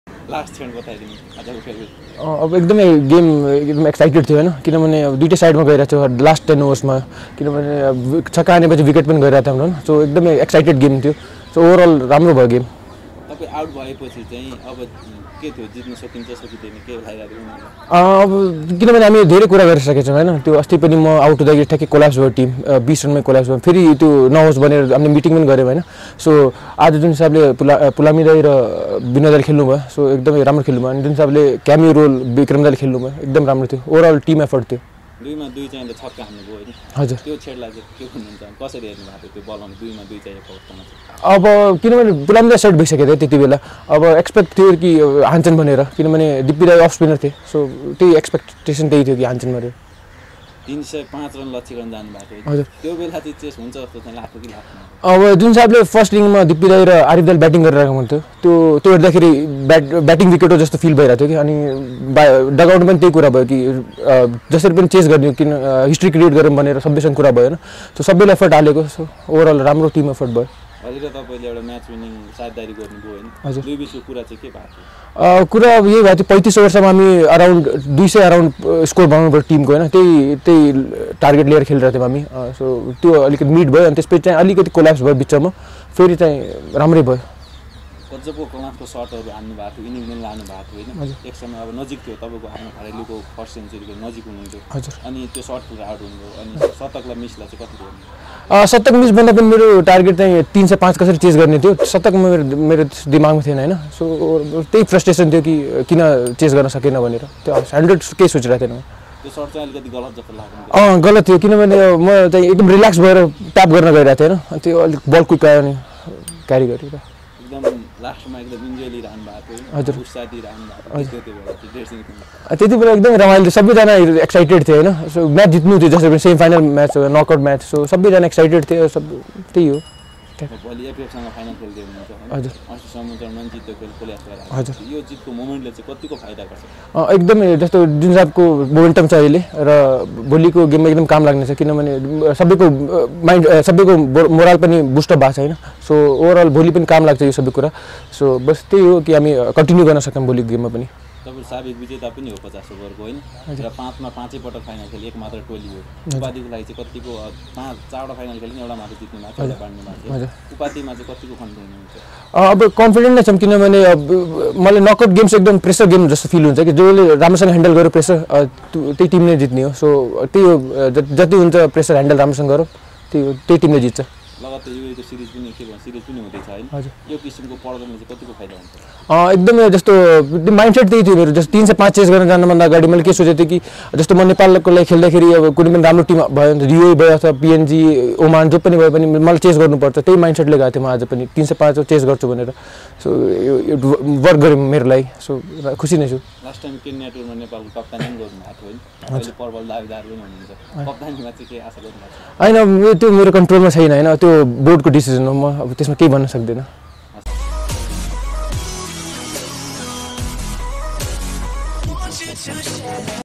'I was frustrated about not finishing the game, never thought century' - Rohit Paudel | रोहित पौडेल
Rohit Paudel of Tribhuwan Army Club speaks after thrilling semifinal win over Nepal Police Club in PM Cup Men's National Cricket Tournament 2079.

Uploading this video on other YouTube channels or Facebook pages or any other platforms without prior permission is strictly prohibited.